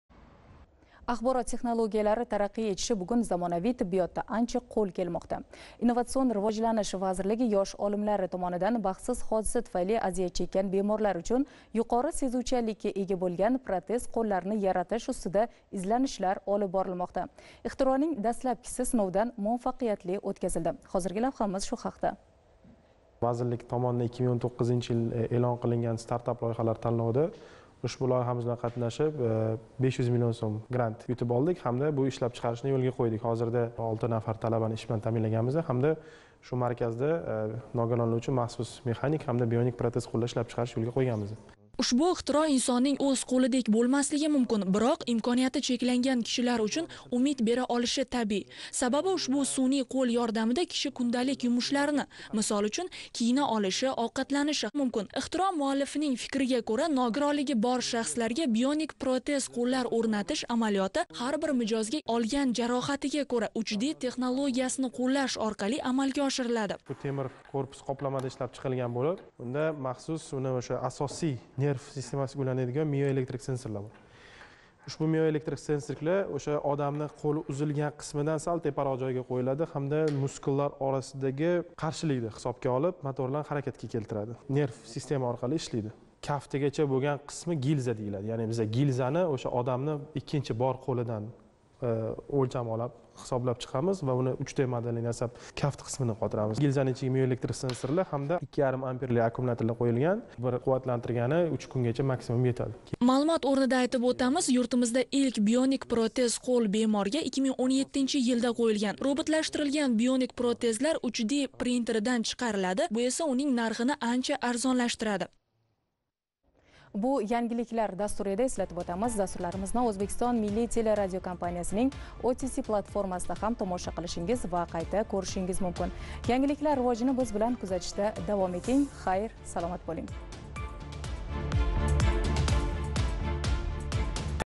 Protez qo‘llarni yaratish bo‘yicha izlanishlar
Innovatsion rivojlanish vazirligi yosh olimlari tomonidan baxtsiz hodisa tufayli aziyat chekkan bemorlar uchun yuqori sezuvchanlikkka ega bo‘lgan protez qo‘llarni yaratish bo‘yicha izlanishlar olib borilmoqda.

"Vazirlik tomonidan 2019 yilda eʼlon qilingan startap loyihalar tanlovida ushbu loyiha bilan qatnashib, 500 mln so‘m miqdorida grant yutib oldik, hamda bu ishlab chiqarishni yo‘lga qo‘ydik. Hozirda 6 nafar talabani ish bilan taʼminlaganmiz. Bu markazda nogironlar uchun mahsus mexanik va bionik protez qo‘llar ishlab chiqarishni yo‘lga qo‘yganmiz", -, deydi ixtirochi Firdavs Nematzoda.